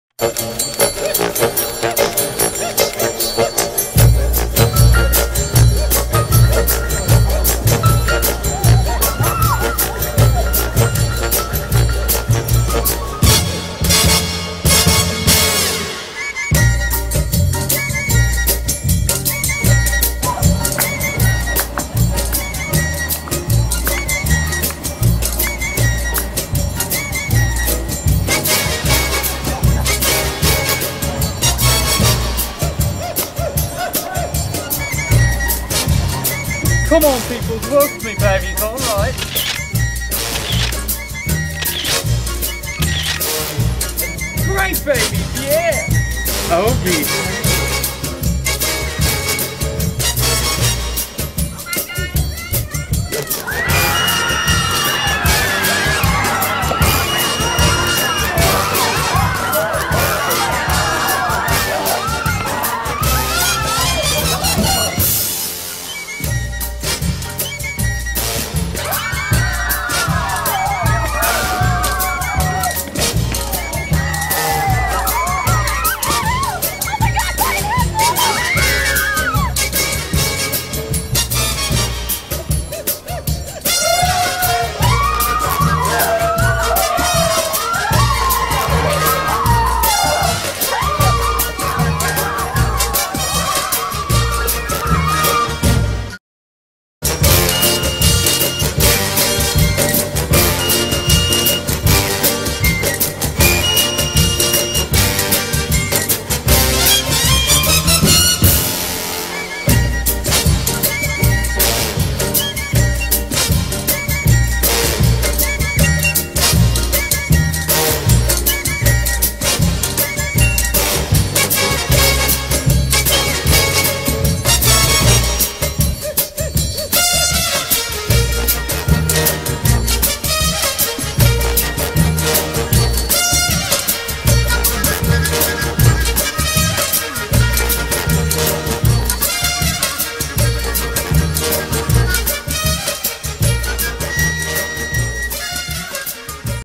Ryan Hedblom for President!
Ohhh BEHAVE! Ryan is back for another run, this time for President of the Student Body. Share this shagadelic piece of cinema to show your support! Thanks to everyone who helped out!